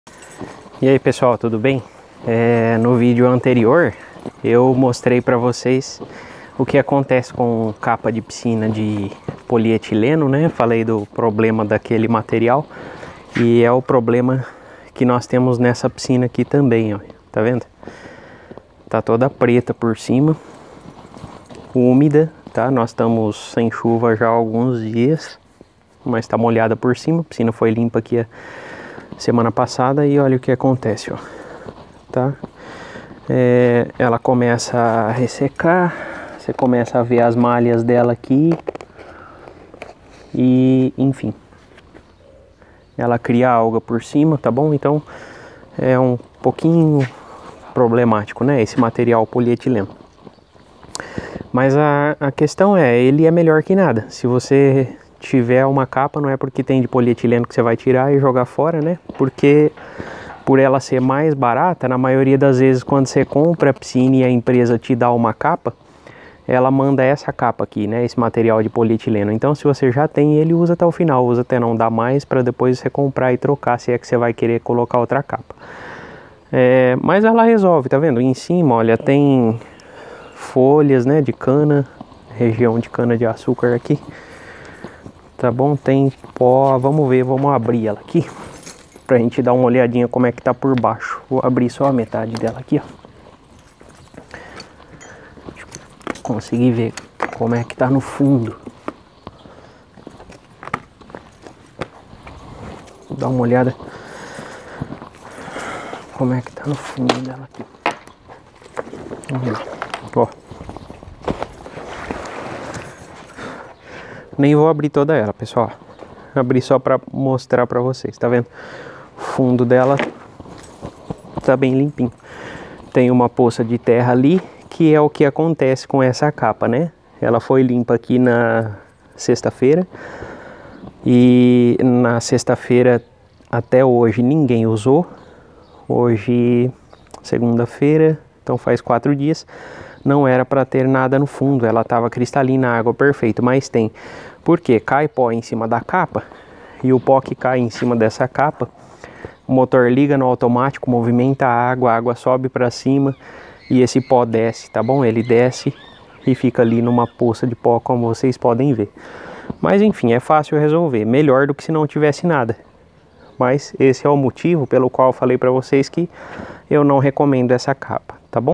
Mesmo com a capa ruim...
Acesse nosso curso atualizado - Piscina Limpa e Saudável 👇🏻👇🏻🚀🚀
A capa de proteção para piscina alivia bastante no tratamento da água.

Algumas capas duram mais, outras menos...

Mas mesmo a capa estando ruim, veja o resultado de uma piscina com capa no vídeo... por isso eu prefiro que tenha capa de proteção nas piscinas.

Conheça nosso curso para tratadores e proprietários de piscina.